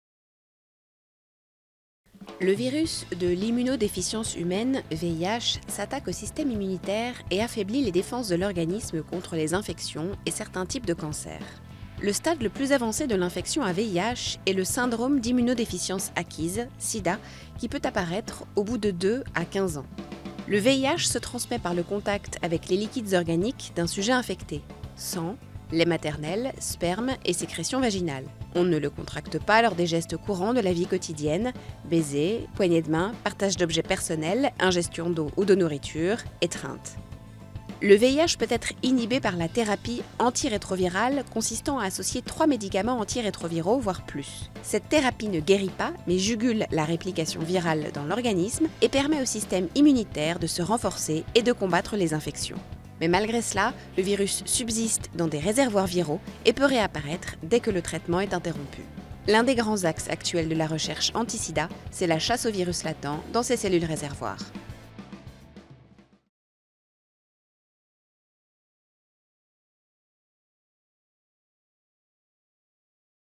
VIDÉOGRAPHIE - comment le virus du VIH agit-il dans le corps ?
Des chercheurs ont confirmé qu’un deuxième patient atteint du sida s’était remis de cette maladie.